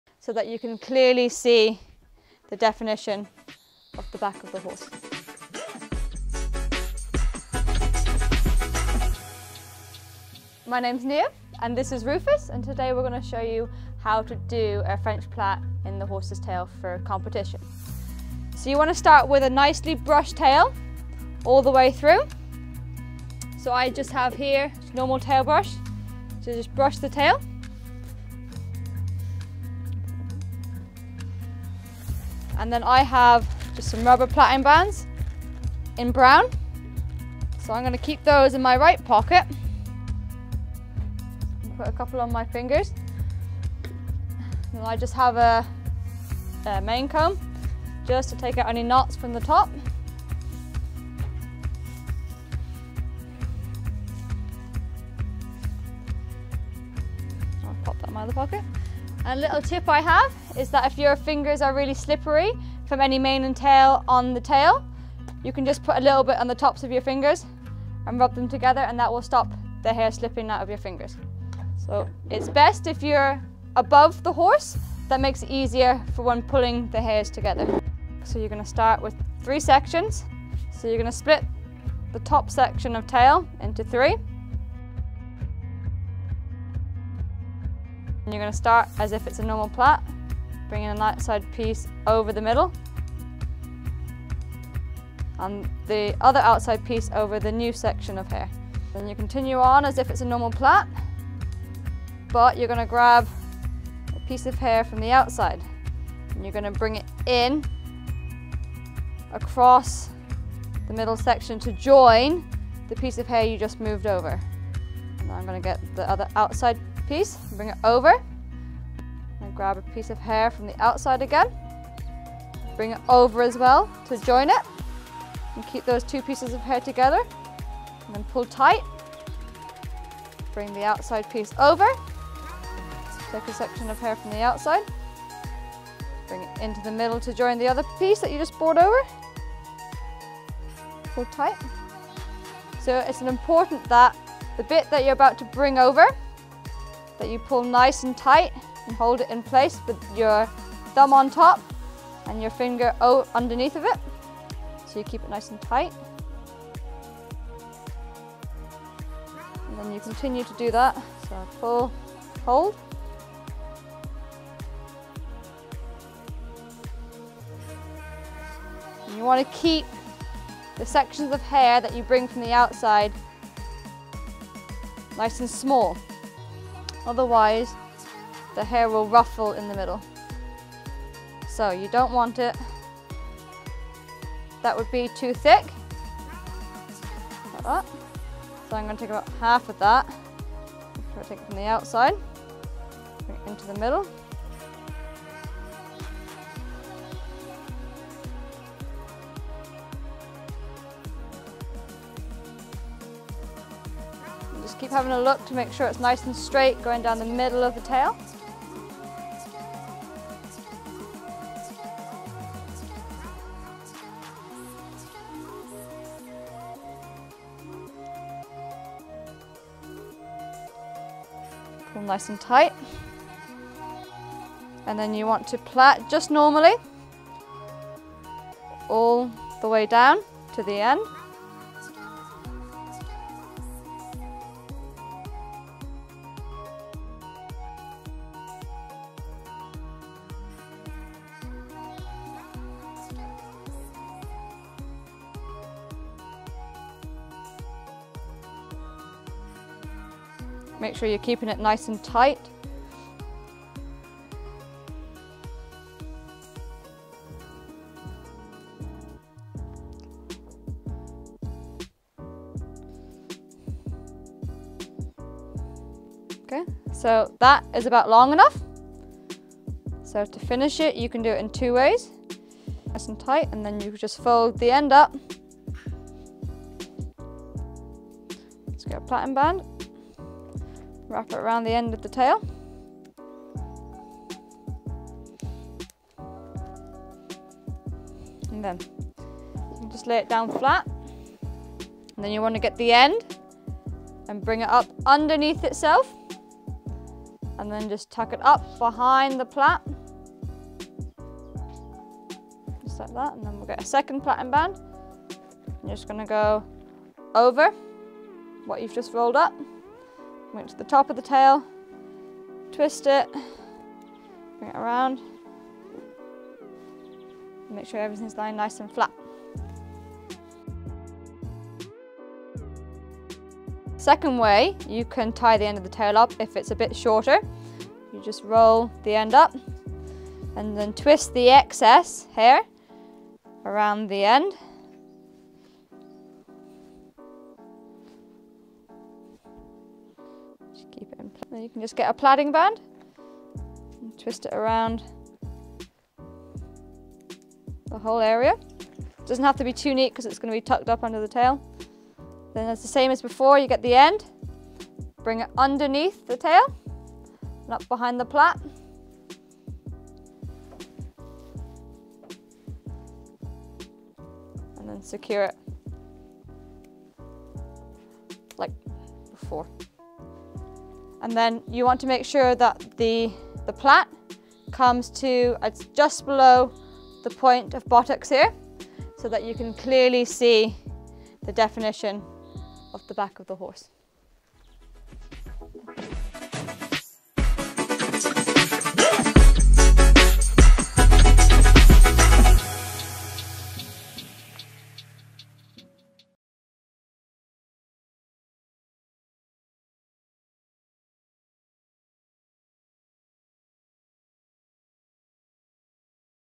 How to plait your horse's tail - Tutorial
How do you plait your horse's tail? This video shows you how to plait you horse's tail for a competition.